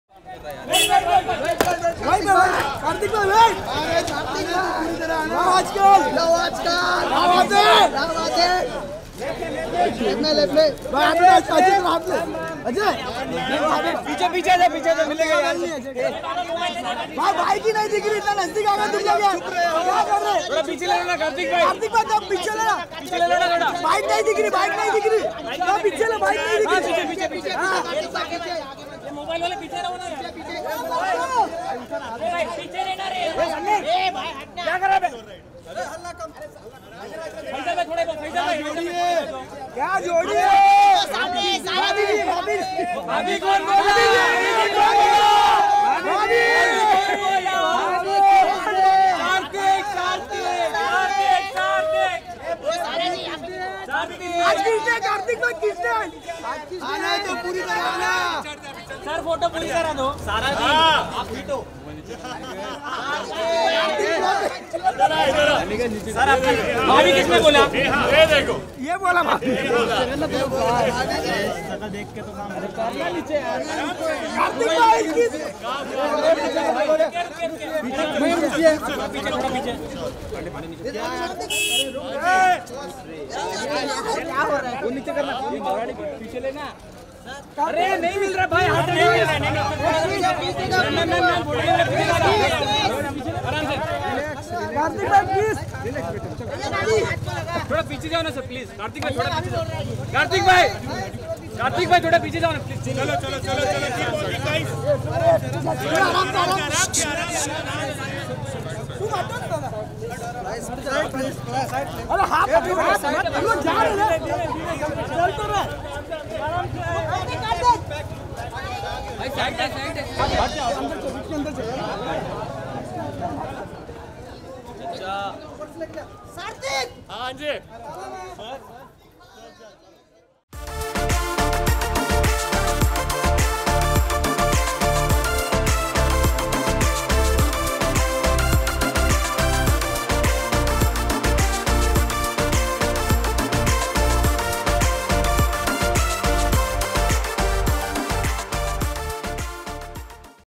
Paparazzi Call Sara Ali Khan Bhabhiji During The Midnight Screening Of Love Aaj Kal | LehrenTV
Kartik Aaryan and Sara Ali Khan arrive of midnight screening of Imtiaz Ali's Love Aaj Kal 2 on bike. Sara blushes as she is called 'Bhabhi' by the media.